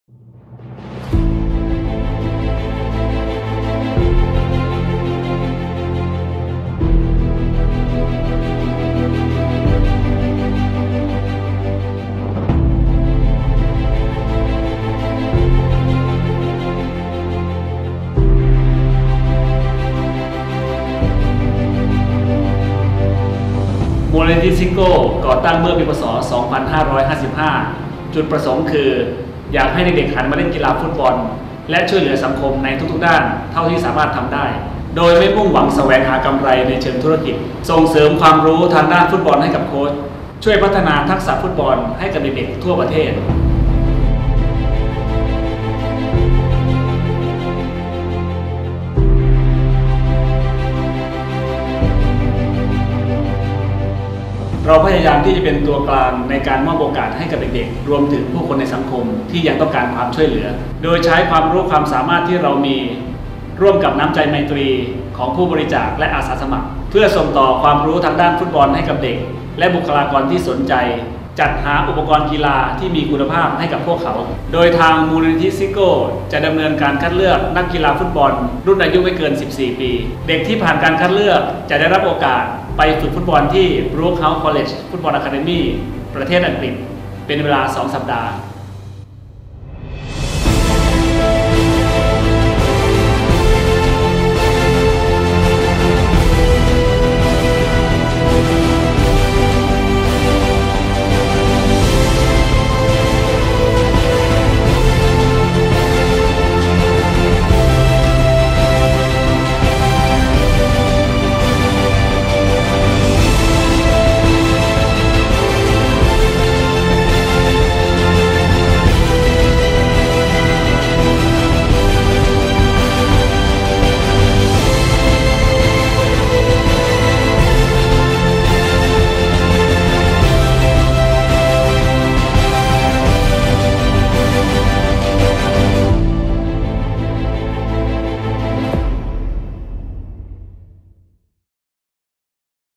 downloadใบสมัคร เล่นฟุตบอลจนได้ดี ไปอังกฤษฟรีกับโค้ชซิโก้
"เล่นฟุตบอลจนได้ดี ไปอังกฤษฟรีกับโค้ชซิโก้ " ปีที่ 2 นี้ เฟ้นหาเยาวชนฝีเท้าดี 15 คน ไปเรียนฟุตบอลและภาษาที่ Brooke House College Football Academy เป็นเวลา 2 สัปดาห์ ฟรีไม่มีค่าใช้จ่าย

ปิดรับสมัคร 8 กรกฎาคม หรือเมื่อเต็มครับ